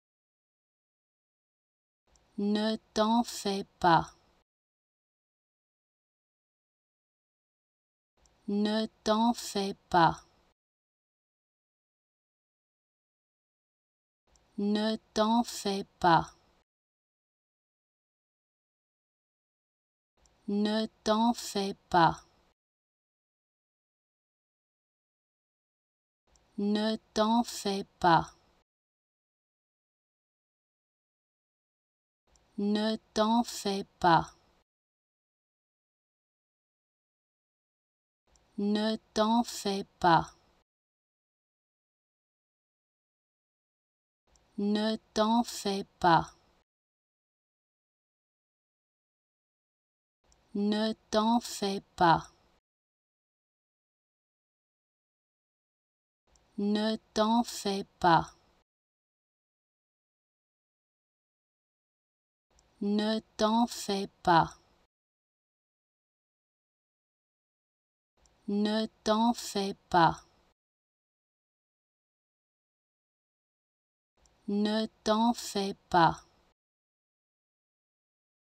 How to Pronounce ''Ne t'en fais pas'' (Don't worry about it) Correctly in French
Learn how to say and properly pronounce ''Ne t'en fais pas'' in French with this free pronunciation tutorial. Apprenez à prononcer des mots en français grâce à nos tutoriels.